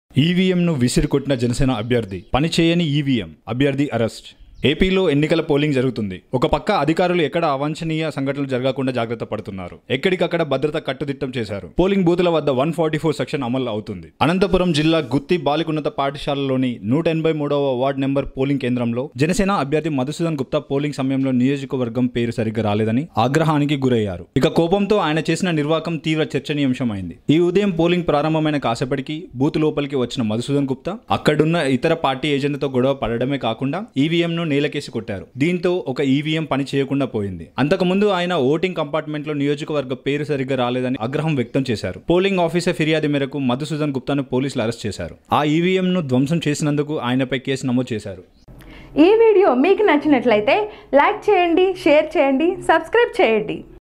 Janasena Candidate Madhusudan Gupta Destroys EVM | Star Dreams Media
Star Dreams Media is the only channel which entertain you and your total family with comedy, sentiment and love stories.